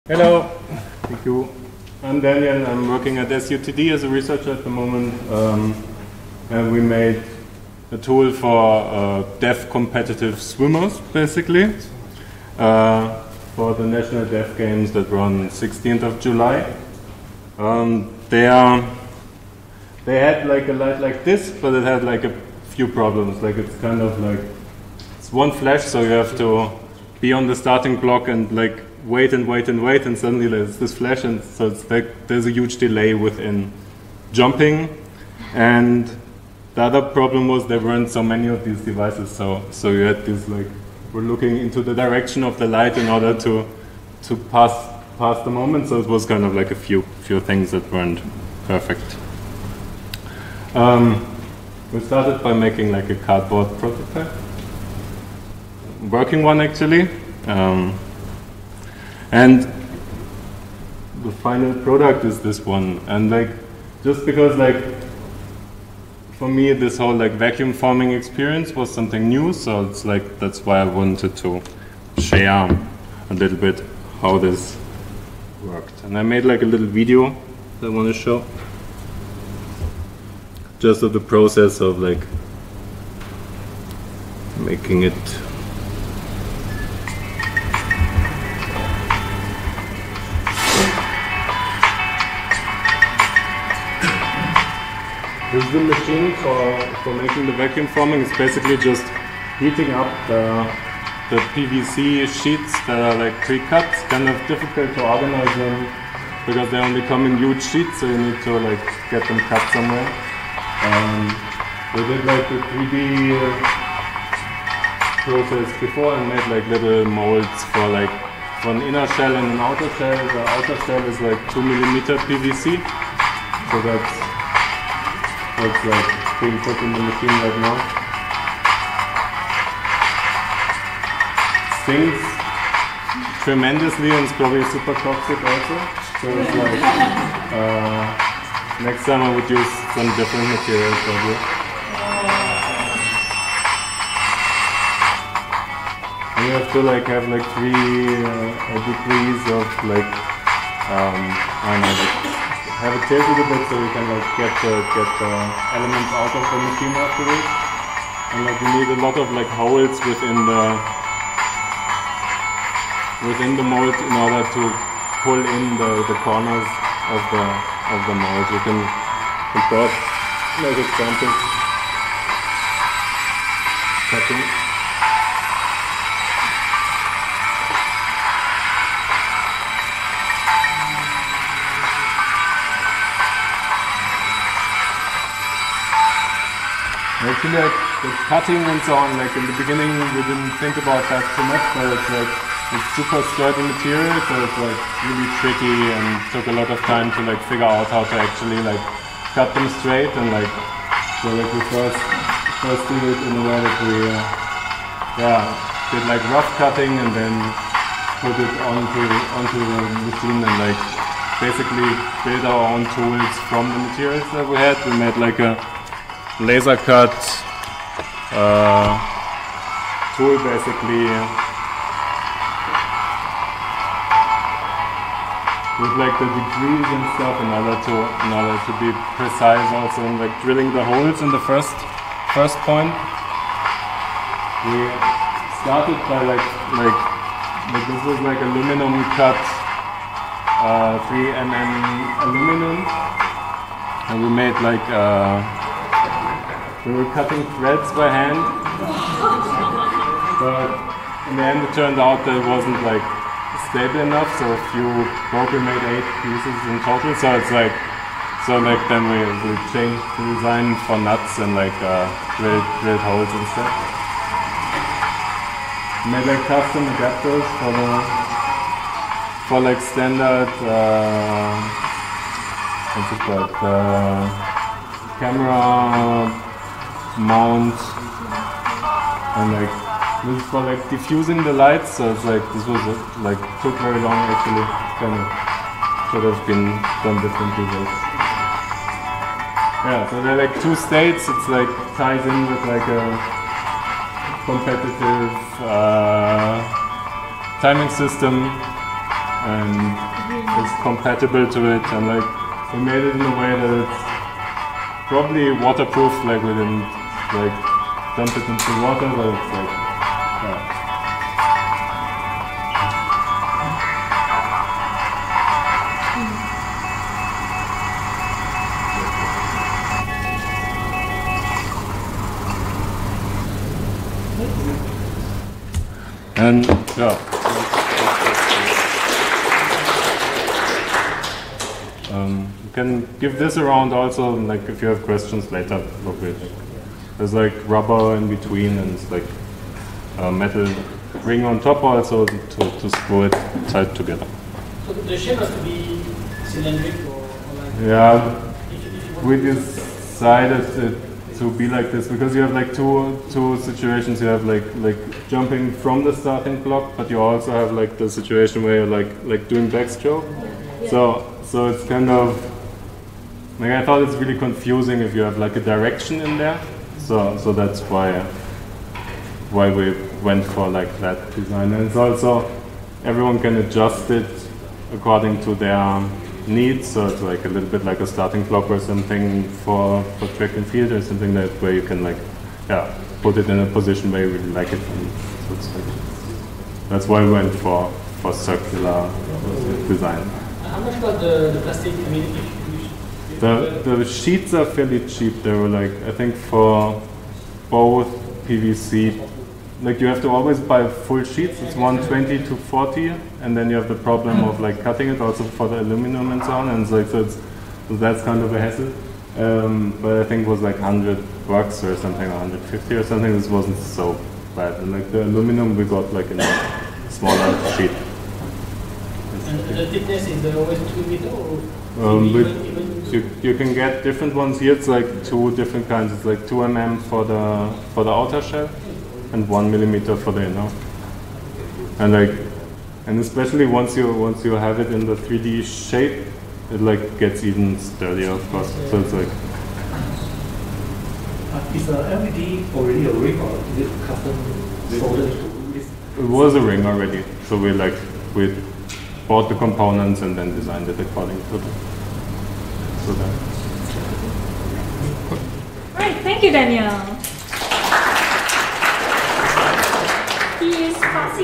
Vacuum forming experiences for deaf competitive swimmers - Hackware
Speaker: Daniel Wessolek